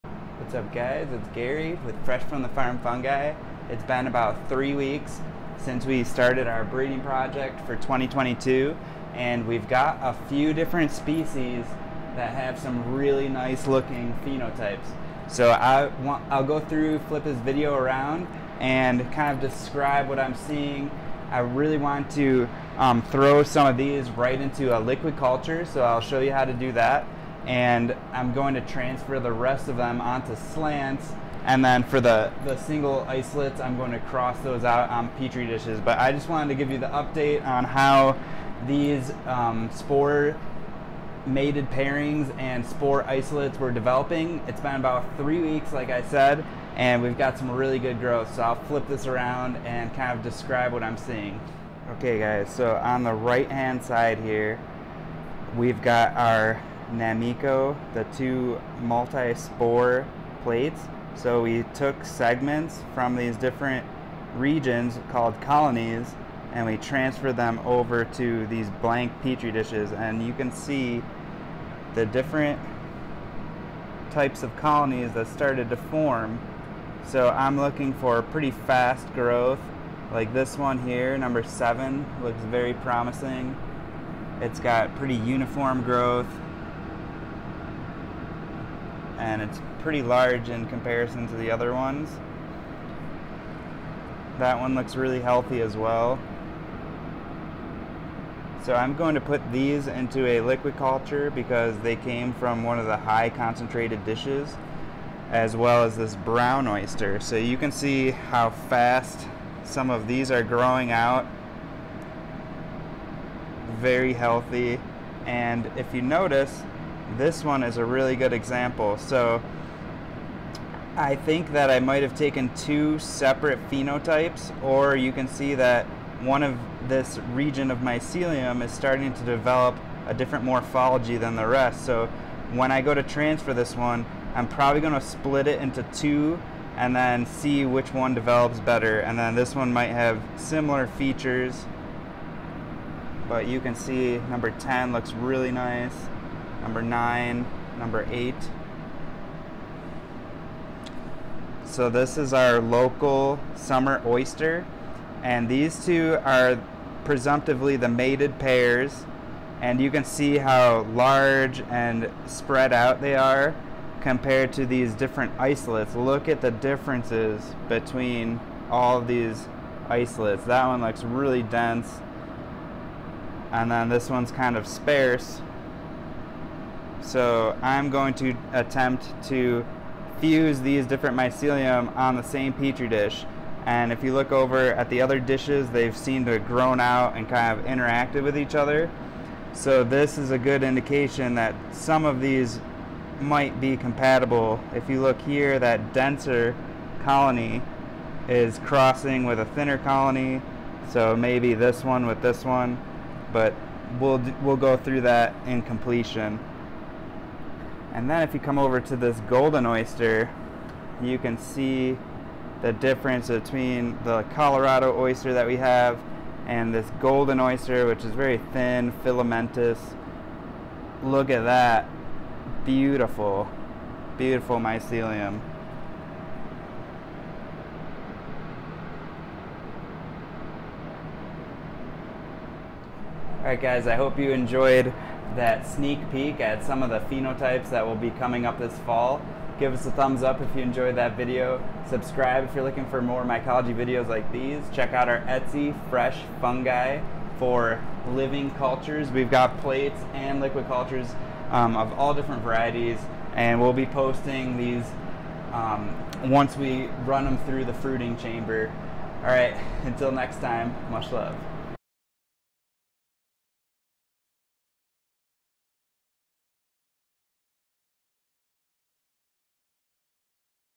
Mycelium on agar - Selecting phenotypes and breeding project updates 2022 - Pheno hunting from spore
This is a three week update on our 2022 breeding project.  We have selected phenotypes of Nameko, Brown Oyster, Colorado Summer Oyster and Golden Oyster and will be describing their nature and the next steps for testing these genetics.  MUSHLOVE

ETSY shop

Gary's favorite lab gear and grow equipment